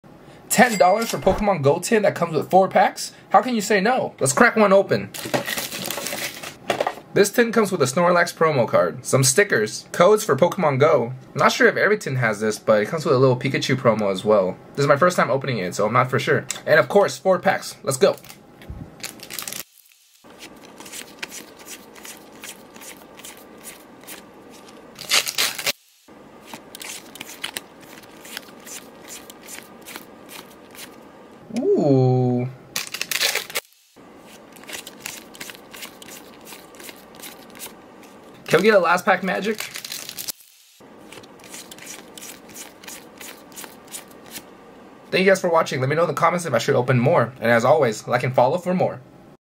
$10 POKÉMON GO TIN OPENING?
Opening up a Pokemon Go tin that I purchased for only $10 at Walmart